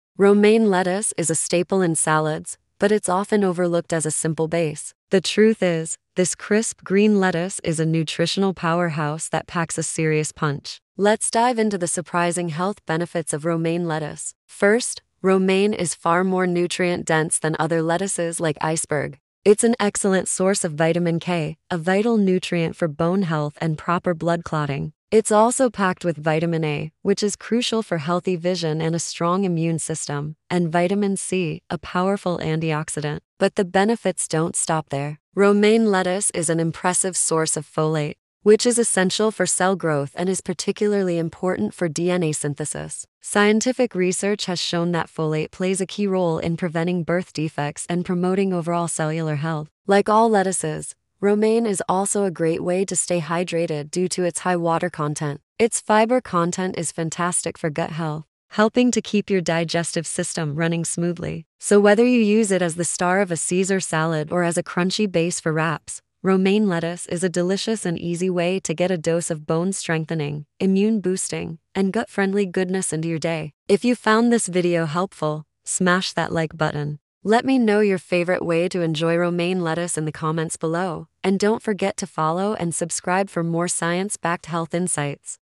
Romaine Lettuce: The Hidden Superpower in Your Salad
Think Romaine lettuce is just a bland base for salads? Think again! In this quick 2-minute video, we'll dive into the incredible science behind this crisp green.
Discover its crucial role as a source of Vitamins K, A, and C, its benefits for gut health, and its importance as a source of folate for overall cellular health.
Get ready to see Romaine lettuce in a whole new light!